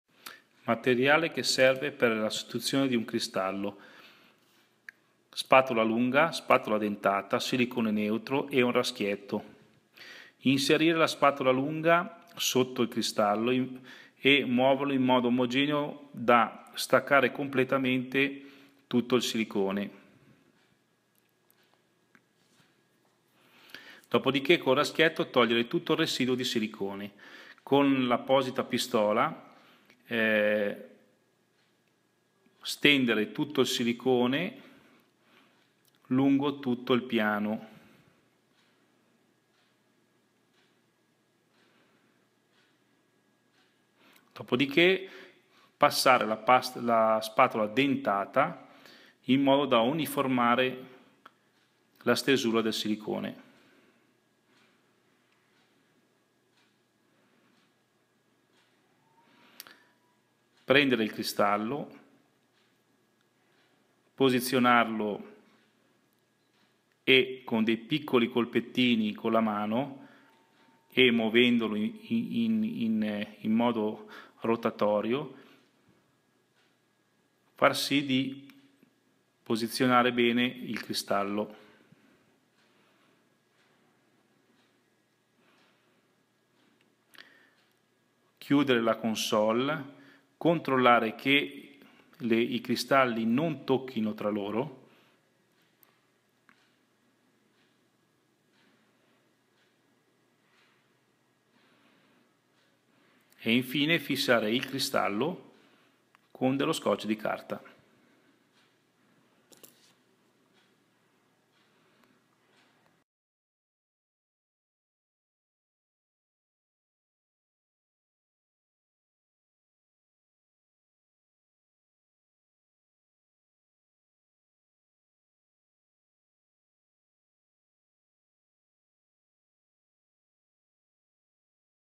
How to replace glass on Ozzio transformable furniture
This video describes how to replace glass lamination on one of Ozzio transformable tables.